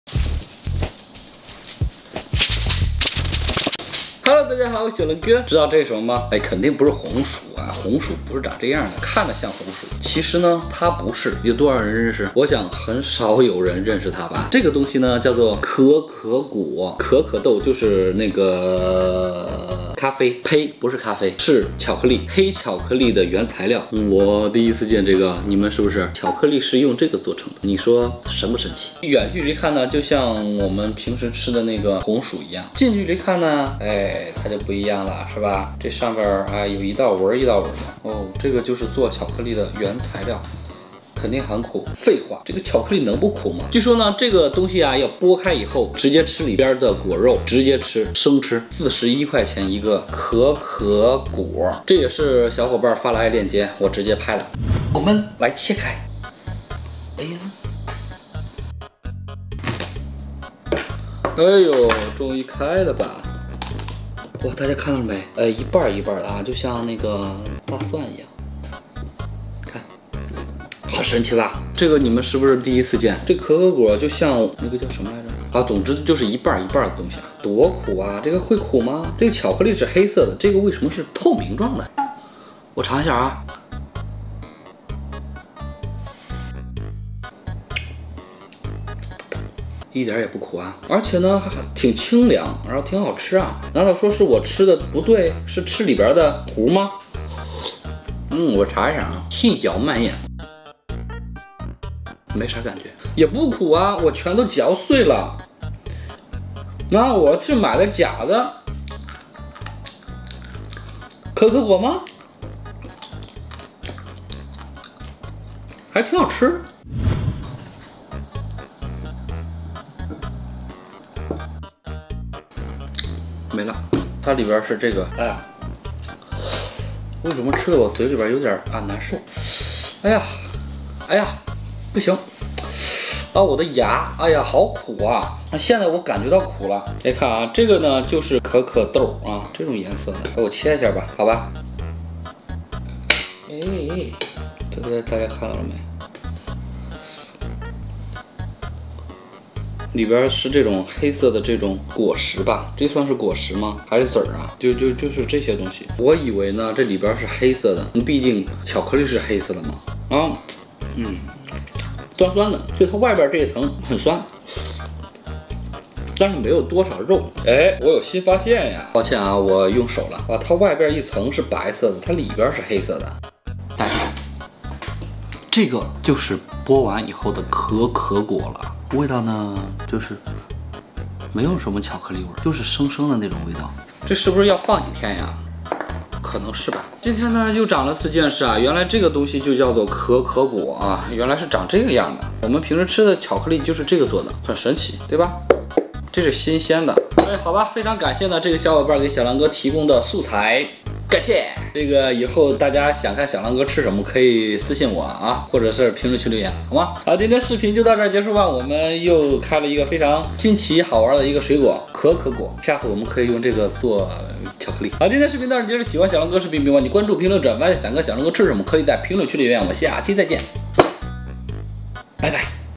生吃巧克力的原材料“可可果”，感觉得有点不一样，我买错了吗？
喜欢视频，请按赞、分享哦~